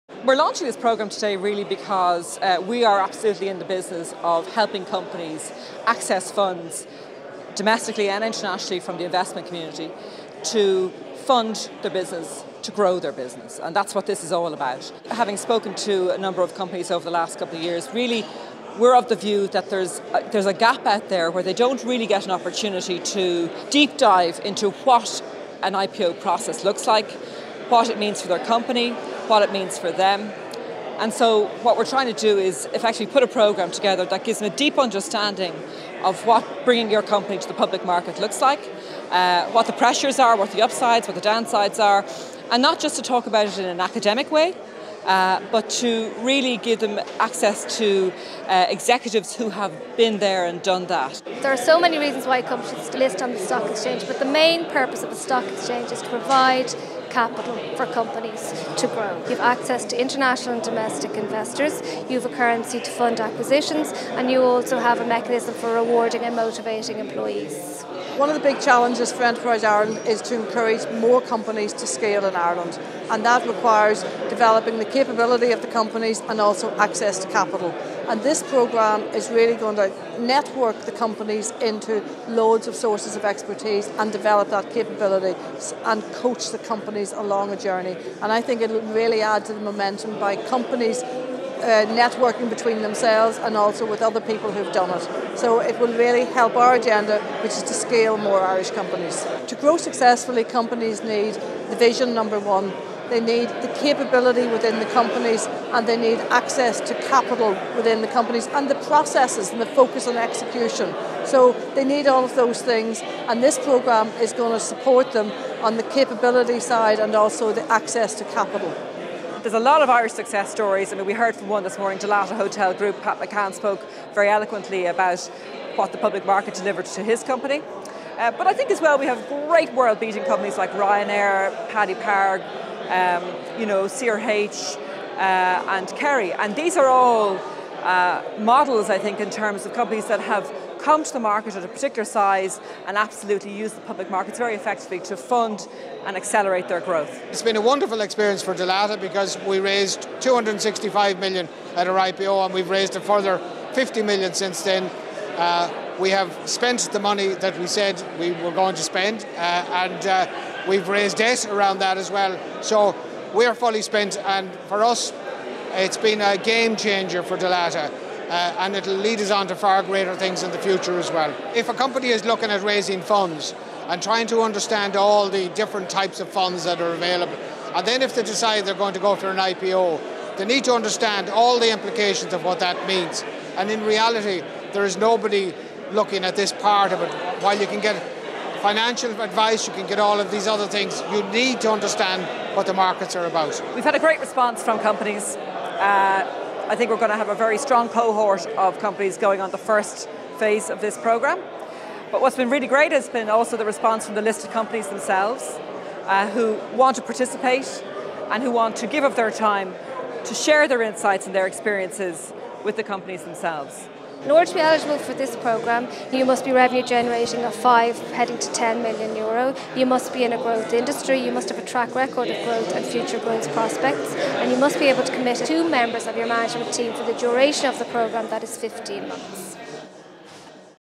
Irish Stock Exchange launches mentoring programme for IPO candidate companies
Video from the launch event for the IPOready programme from the Irish Stock Exchange